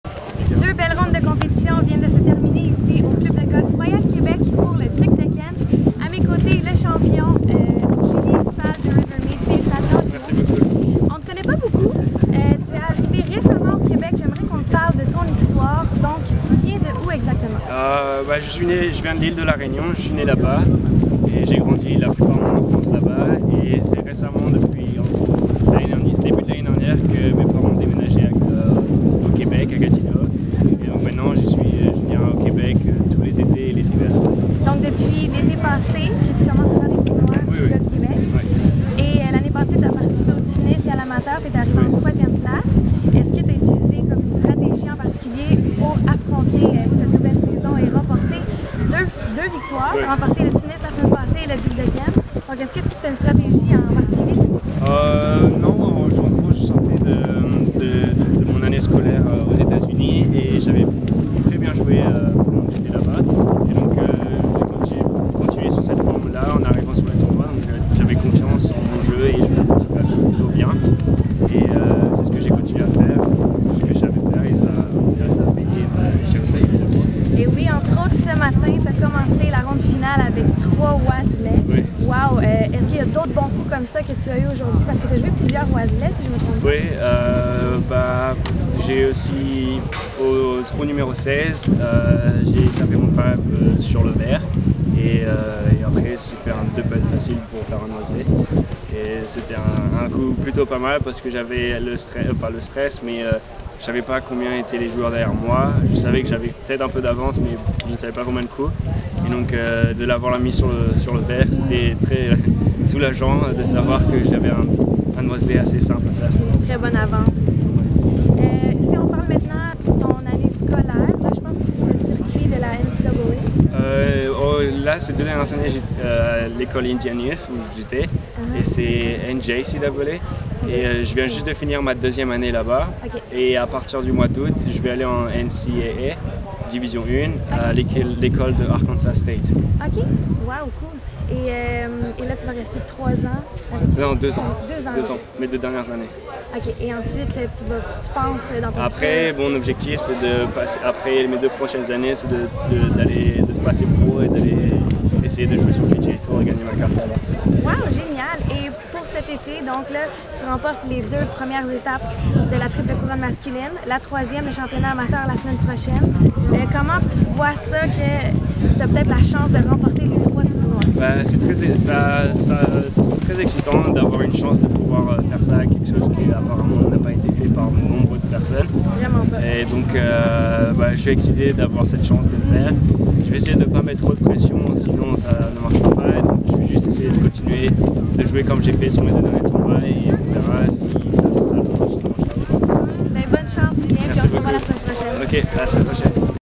Duc de Kent 2018 | Entrevue avec Julien Sale
Entrevue avec le champion du Duc de Kent 2018, Julien Sale de Rivermead.